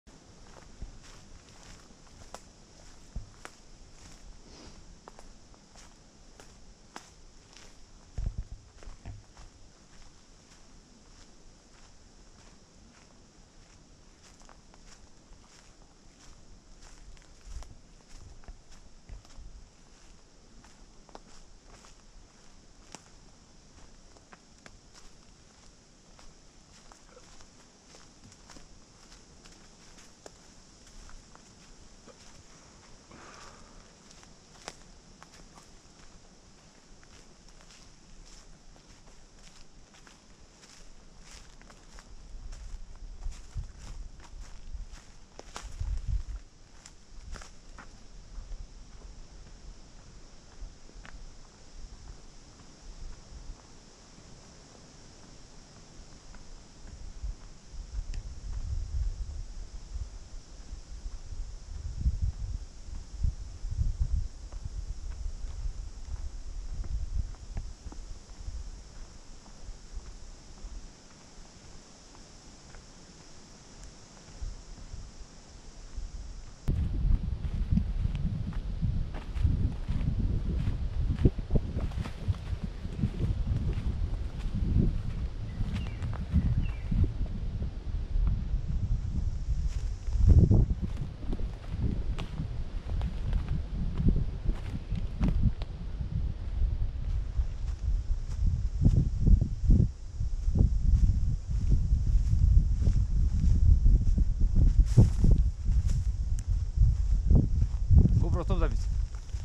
VR 360. Березовая роща. Birch grove.
Березовая роща. VR 360.
Для погружения смотреть с телефона, в стандартном приложении Youtube.
Находится роща недалеко от Партизанского водохранилища, на территории заповедника.